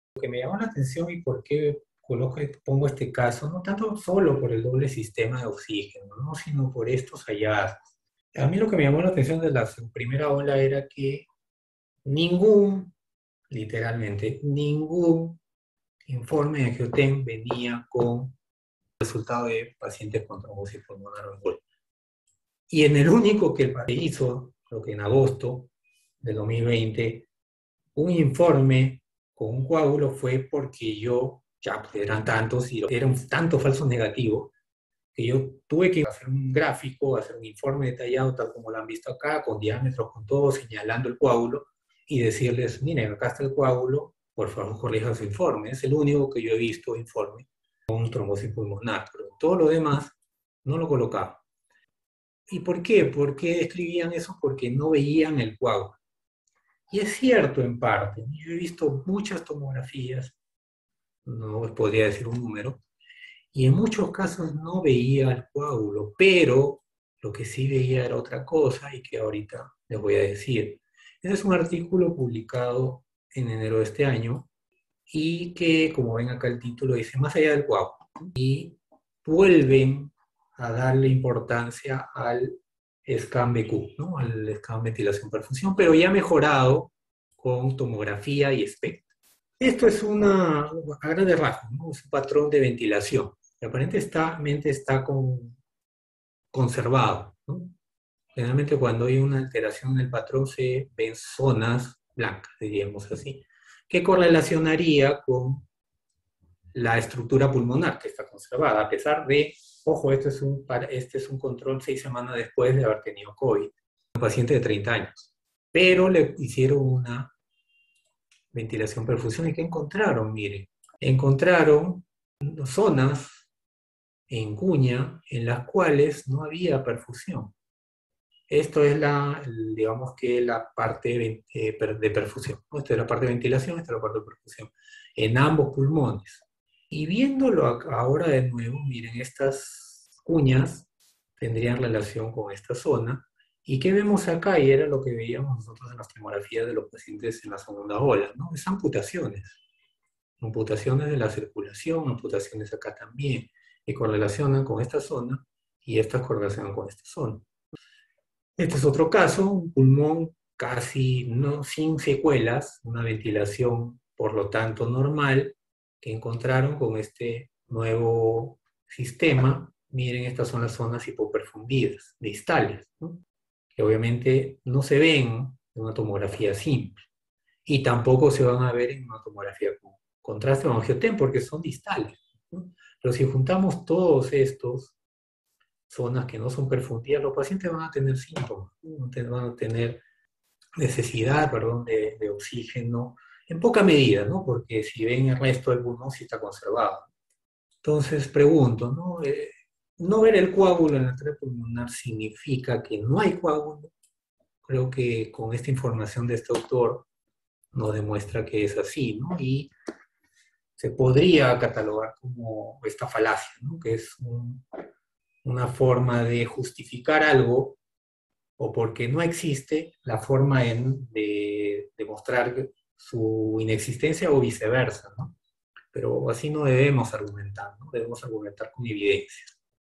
Covid-19 y Trombosis pulmonar: Más allá de la angioTEM
Un artículo de Dhawan RT M.D y colaboradores publicado a principios del año 2021 que muestra la falta de perfusión pulmonar que “no vemos” en la mayoría de angioTEM en pacientes con neumonía Covid-19, no severa.
Me pregunto, cómo serán los scan ventilación perfusión en los casos de neumonía severa por covid-19 en los que más dl 80% de ambos pulmones son totalmente blancos?

Dhawan RT, Gopalan D, Howard L, et al. Beyond the clot: perfusion imaging of the pulmonary vasculature after COVID-19. Lancet Respir Med. 2021 Jan;9(1):107-116. doi: 10.1016/S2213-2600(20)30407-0